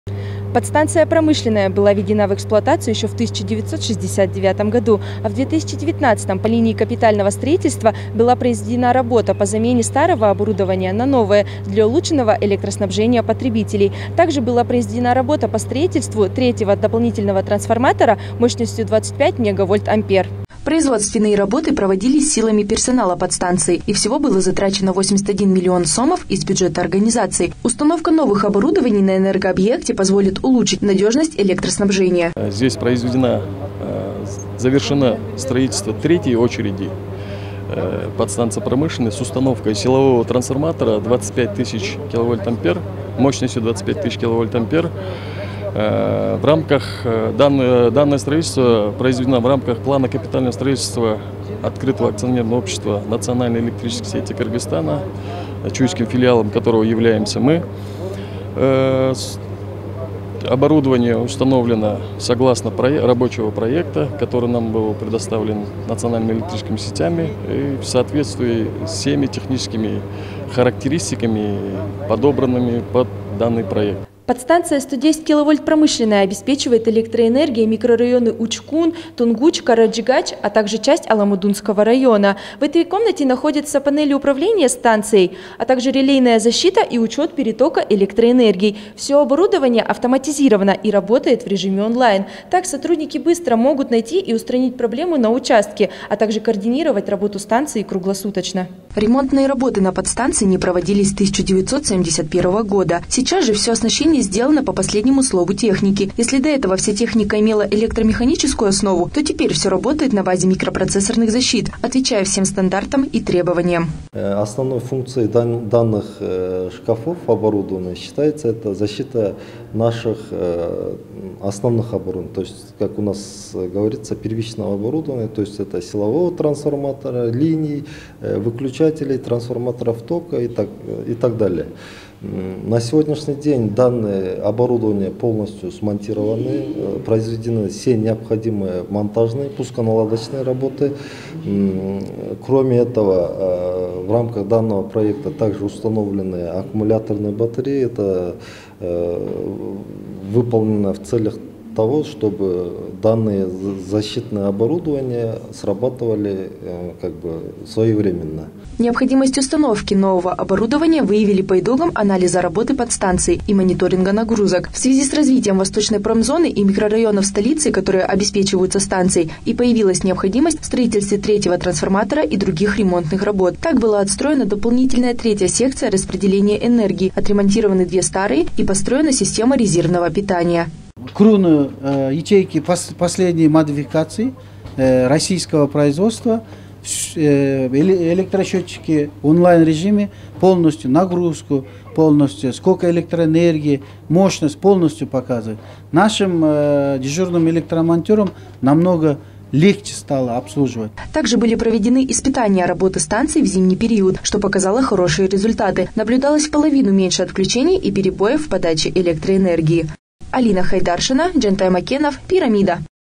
Завершились работы по линии капстроительства подстанции «Промышленная»
Завершились работы по линии капстроительства подстанции в 110 киловольт «Промышленная»